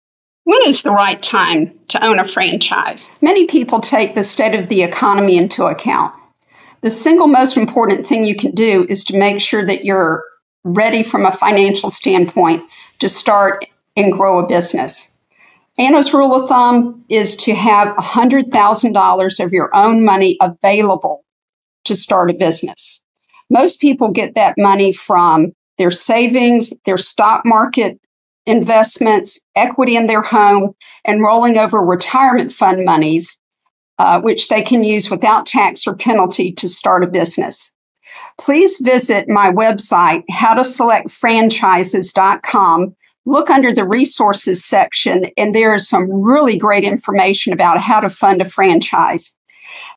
When Is The Right Time To Open A Franchise?
When is the right time to own a franchise? A lot of people are worried about the economy, which political party is in power, and various world events like what’s happening in Ukraine and China.

Here is my advice as to when the time is right for owning a franchise: when your financial house is in order. What does that mean?

- Your credit report is cleaned up and your credit score is good (above 740).
- Your living expenses are covered for the next year.
- You have at least $100,000 of non-financed money to invest.

Here are the main sources of this liquid capital:

- Savings or money market account
- Stock market investments
- Equity in your home (Home Equity Line of Credit or HELOC)
- Retirement funds (qualified ones can be used without tax or penalty for a business)

Today this important topic was discussed in The Franchise Minute - a weekly webinar that presents one topic in 60 seconds. 15 minutes of Q & A follows.